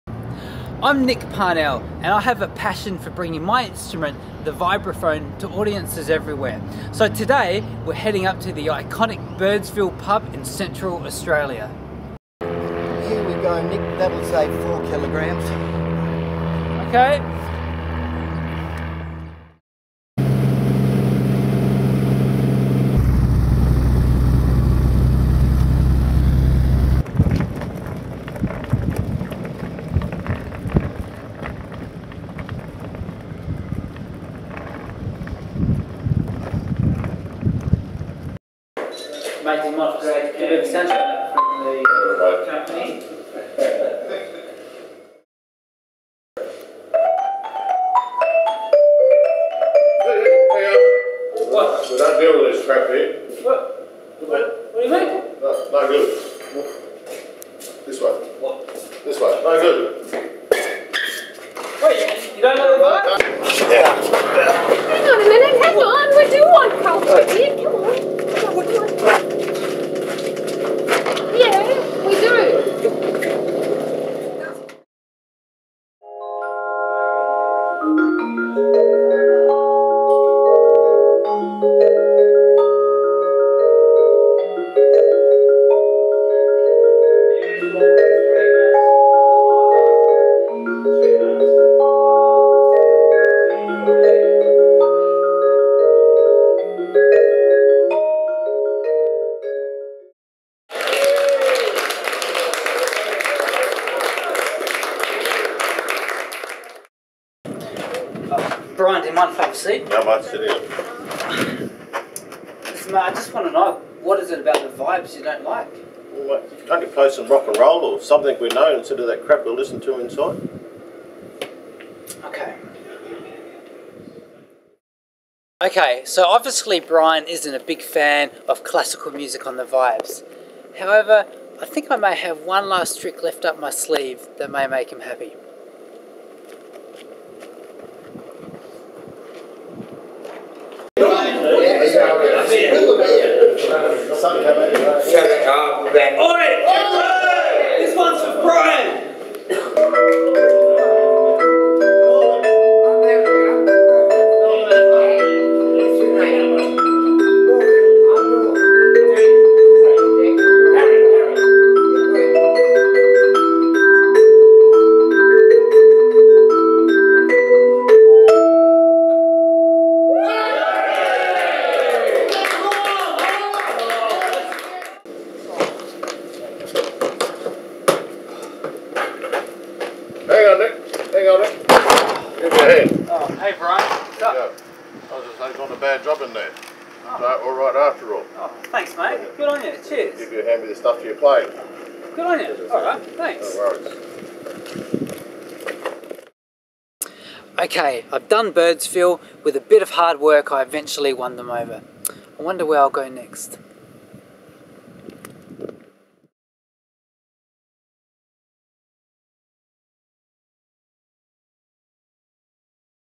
Nick Parnell plays on bar of Birdsville Pub!
Vibes Virtuoso Nick Parnell takes his vibraphone to the iconic Birdsville Pub in outback Australia - plays on bar!